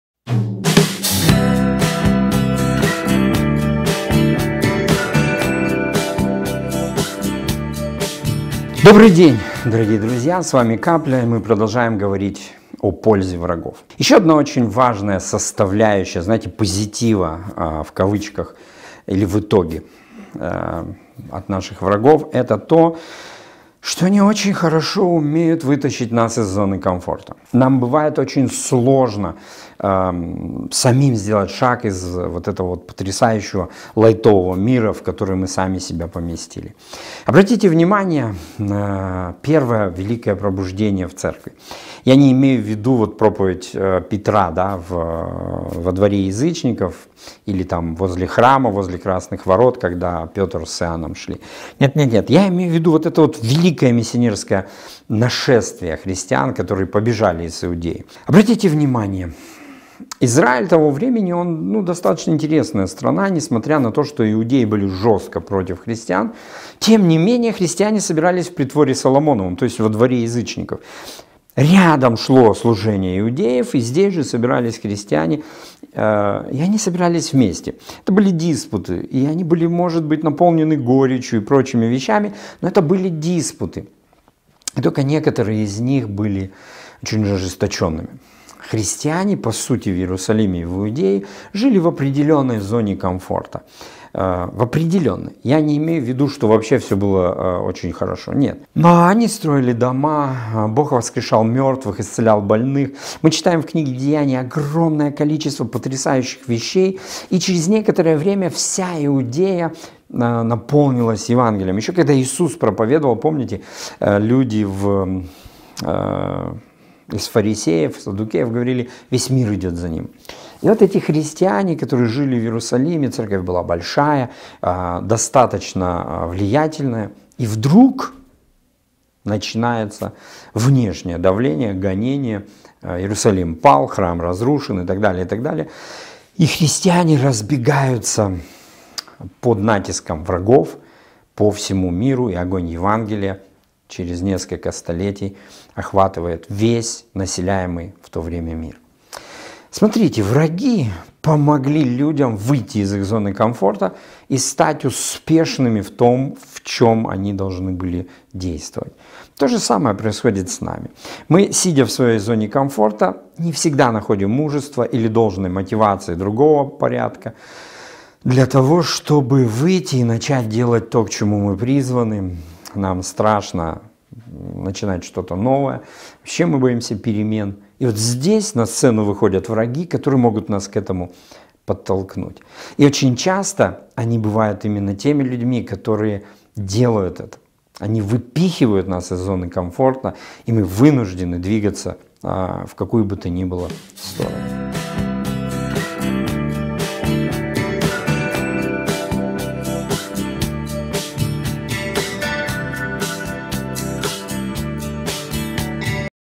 ЗОНА КОМФОРТА
Моральный авторитет отца. Очищение совести. Для развития морального авторитета необходимо быть подотчетным. Враги помогают нам выйти из своей зоны комфорта. Программа «Капля».